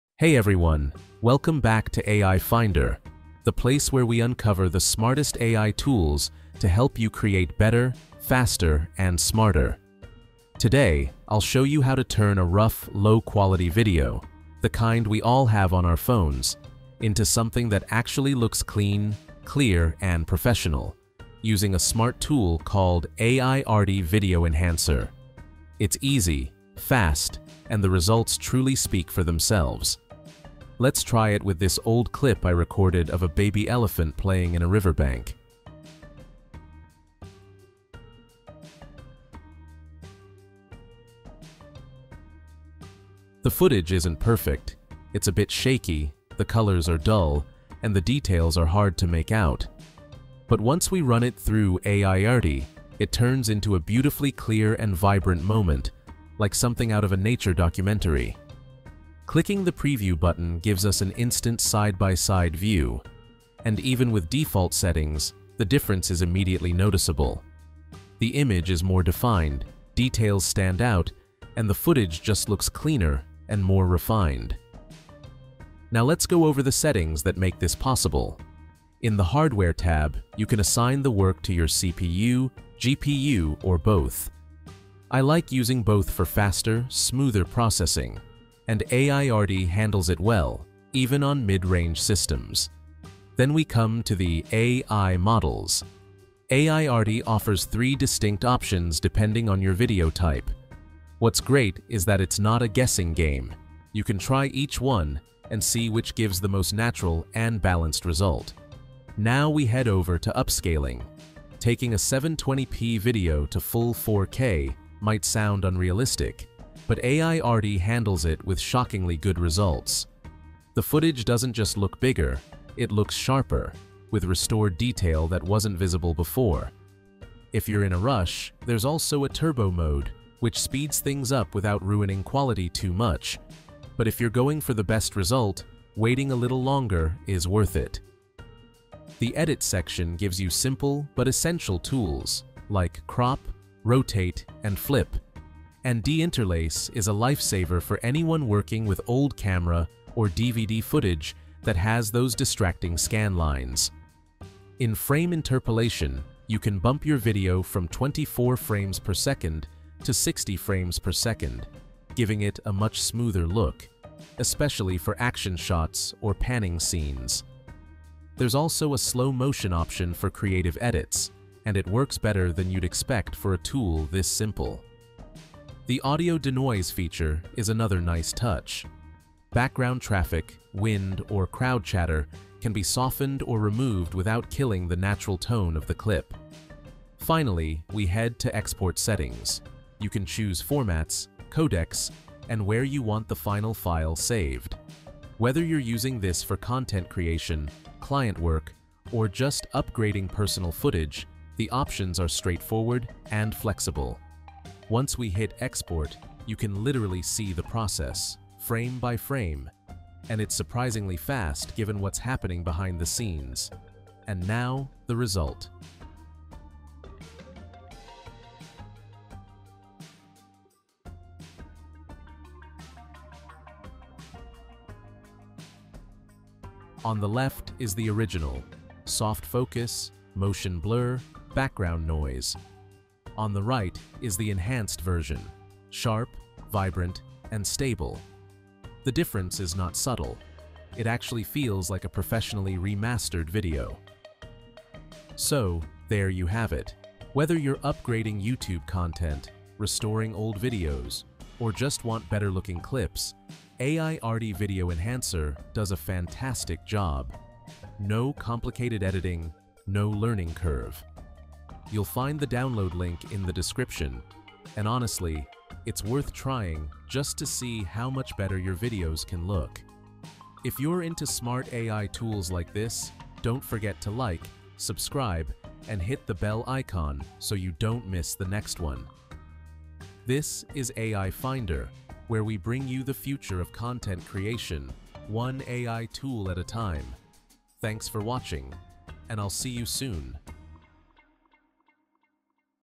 AI Upscale Any Low-Quality Video into Stunning 4K | AIARTY Video Enhancer
What if you could take that shaky, grainy, low-quality video sitting on your phone and instantly turn it into a crystal-clear 4K masterpiece?

In this video, I’ll show you exactly how I transformed a dull old clip of a baby elephant into something that looks like it came straight from a nature documentary — all thanks to an AI-powered Video Enhancer.

We’ll go step-by-step through upscaling, sharpening, color restoration, noise removal, frame interpolation, and even adding buttery-smooth slow motion — all without touching complicated editing software. Whether you’re restoring old family memories, upgrading your YouTube content, or creating cinematic shots from your phone footage, this AI tool will blow you away.